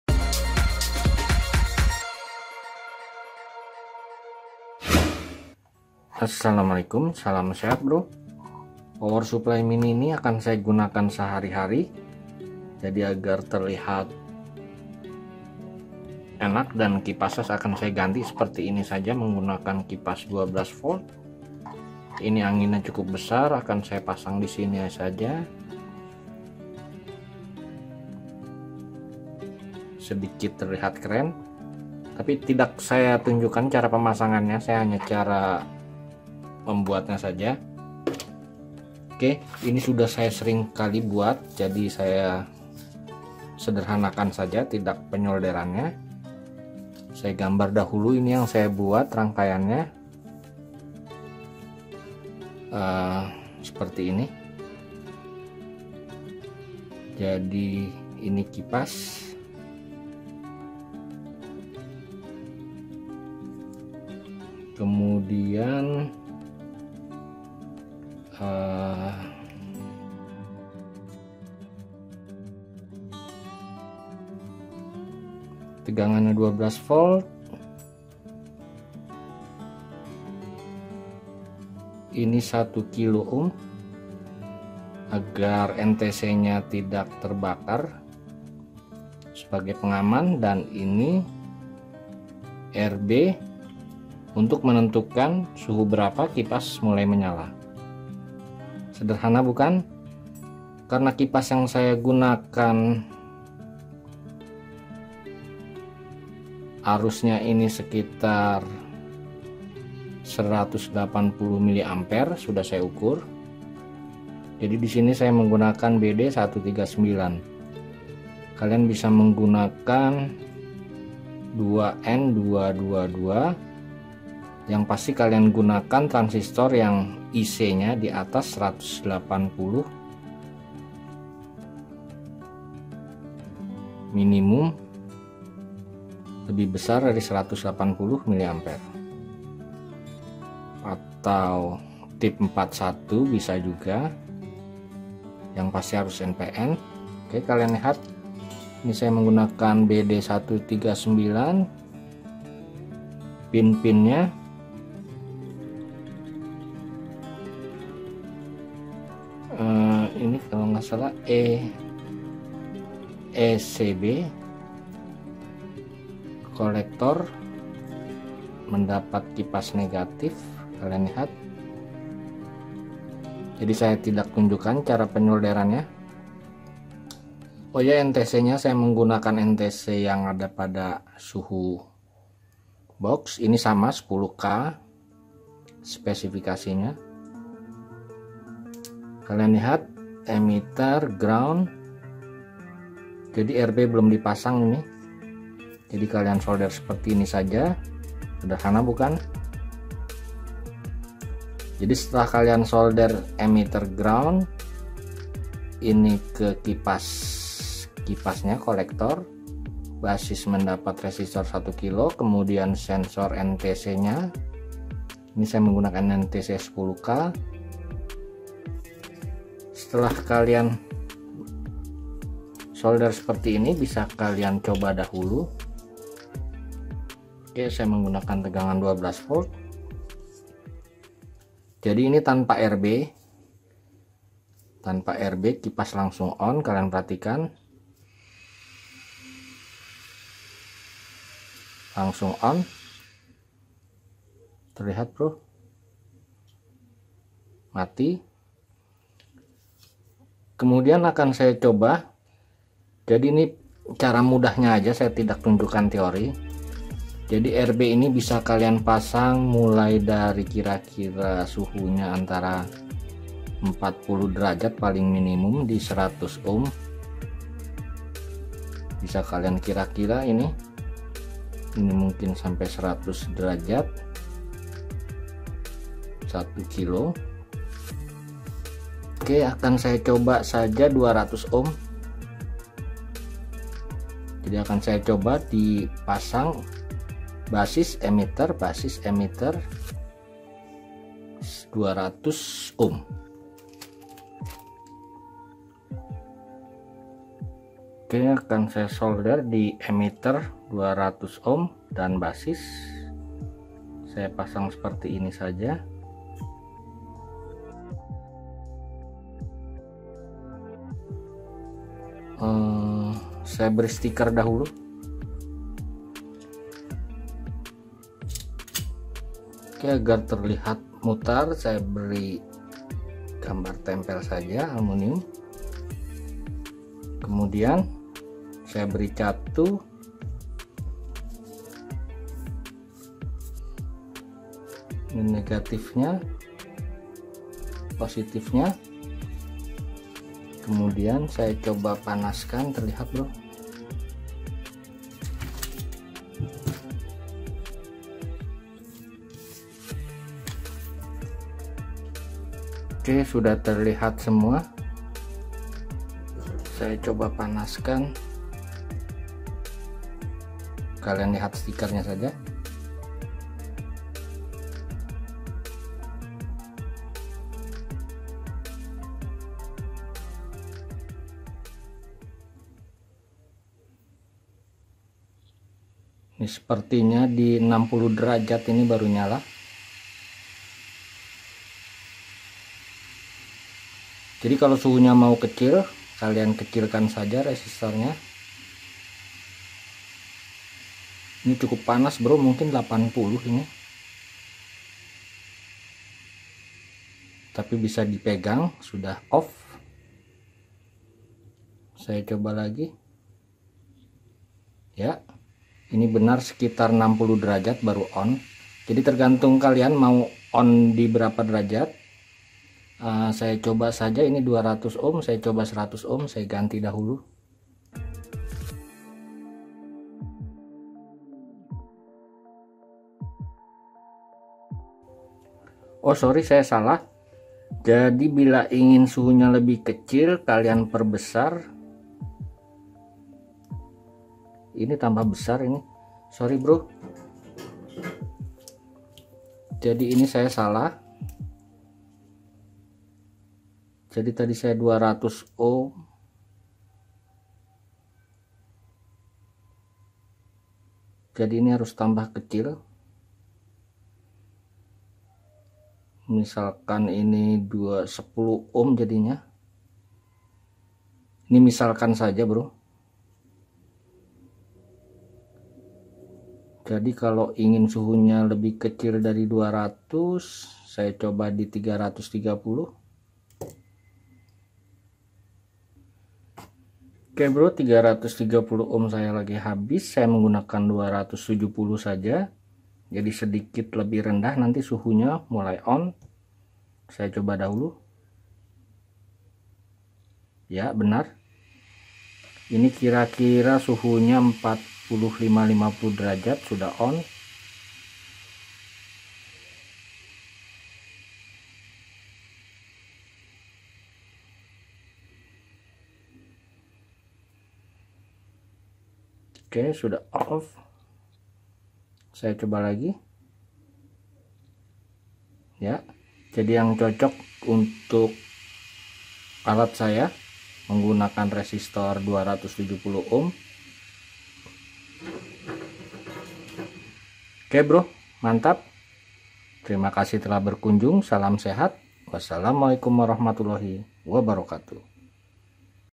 Kipas DC 12V Otomatis ON Off Sensor suhu ➖ Temperatur sensor
Menyalakan Kipas Otomatis Menggunakan Sensor Suhu
Sistem Kipas Angin Otomatis Dengan Sensor Suhu
Video ini menunjukan cara ,membuat kipas pendiin otomatis ON dan Off.

00:00 Radal05
00:12 Mengganti kipas pendingin
00:50 Skematik
04:33 Menentukan Rb, menentukan suhu mulai On
05:20 Test

Kotak / Box Casing Plastik Adaptor Komponen DIY X5 148x97x50 mm